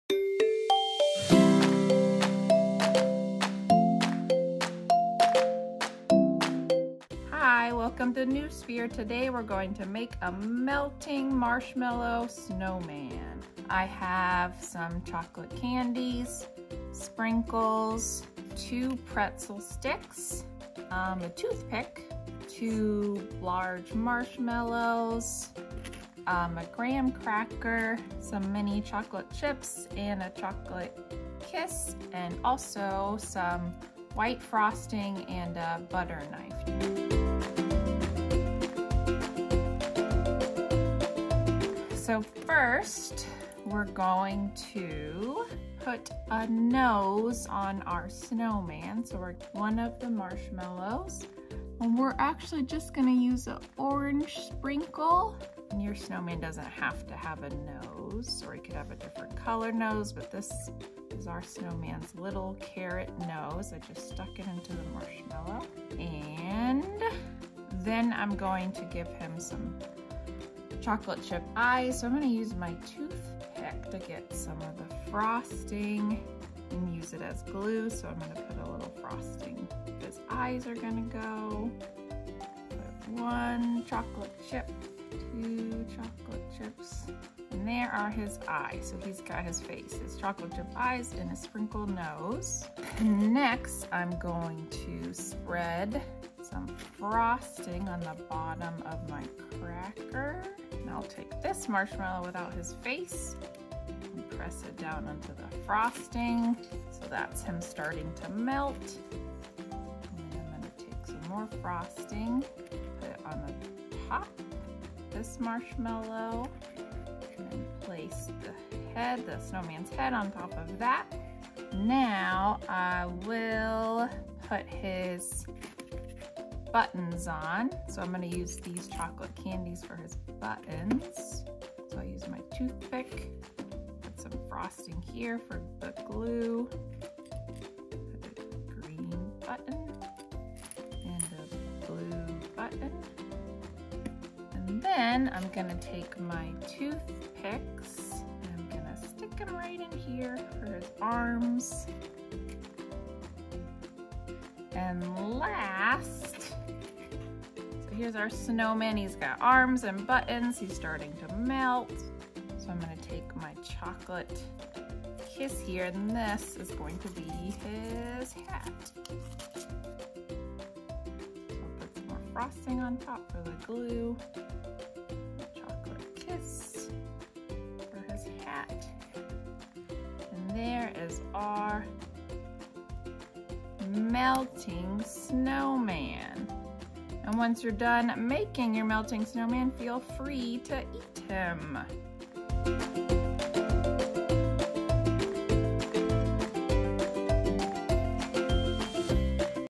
Melting Snowman Snack
In this video, we’ll be making a fun winter treat with your little one. Nothing is more exciting than making a craft and eating it too! Learn how to make a melting snowman snack using a cracker, marshmallows, frosting and candies of your choice. Have fun and enjoy!

Materials:
- Chocolate candies (2)
- Sprinkles (1 long orange piece)
- Pretzel sticks (2)
- Toothpick (1)
- Large marshmallows (2)
- Graham cracker or cookie (1)
- Mini chocolate chips (2)
- Chocolate kiss (1)
- White frosting (or marshmallow fluff)
- Butter knife

Steps:
1. Put an orange sprinkle “nose” in one of the marshmallows. (0:50)
2. Put the chocolate chips “eyes” on the same marshmallow using the toothpick and frosting. (1:09)
3. Spread frosting on the graham cracker using the butter knife. Place a plain marshmallow on the graham cracker. (1:33)
4. Put more frosting on top of the marshmallow using the butter knife. Place the other marshmallow (with the face) on top of the marshmallow on the graham cracker. (1:50)
5. Put two chocolate candy “buttons” on the lower marshmallow using a toothpick and frosting (2:05)
6. Stick two pretzel sticks in the sides of the bottom marshmallow for the “arms”. (1:26)
7. Put the chocolate kiss “hat” on top of the upper marshmallow using frosting. (2:42)
8. Eat and enjoy!